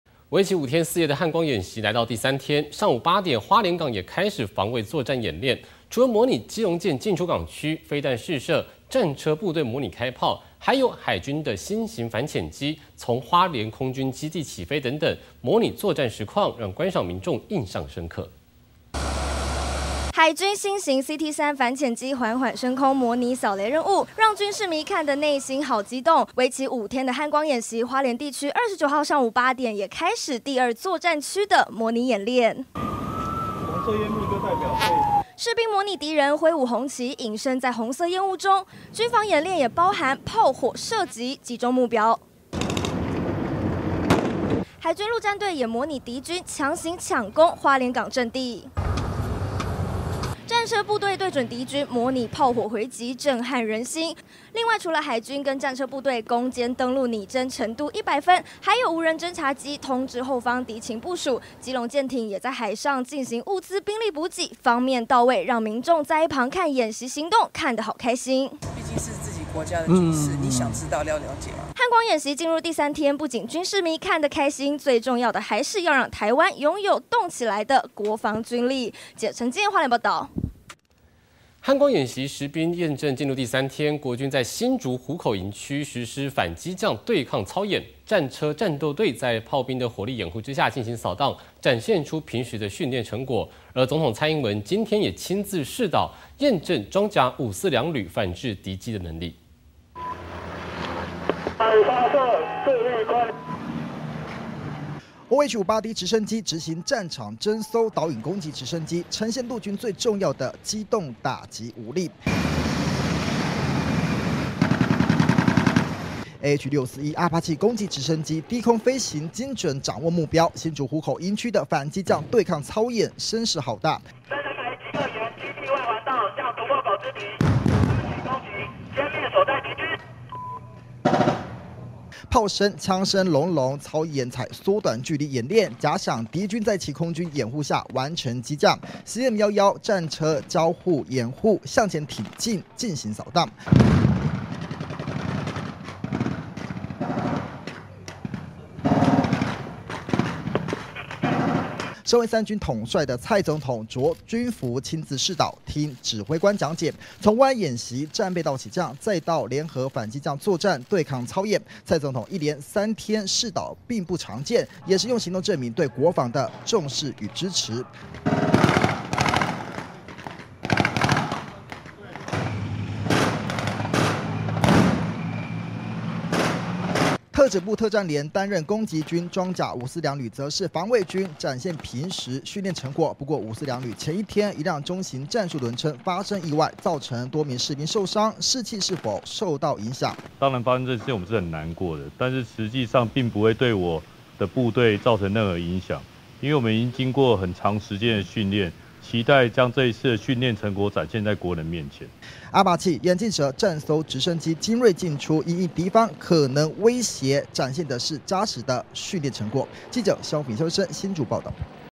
模擬作戰飛彈試射.砲火回擊畫面震撼
為期五天四夜的漢光演習，來到第三天！上午八點，花蓮港也開始防衛作戰演練，除了模擬基隆艦進出港區、飛彈試射、戰車部隊模擬開砲，還有海軍的新型反潛機，從花蓮空軍基地起飛等等，模擬作戰實況，讓觀賞民眾印象深刻！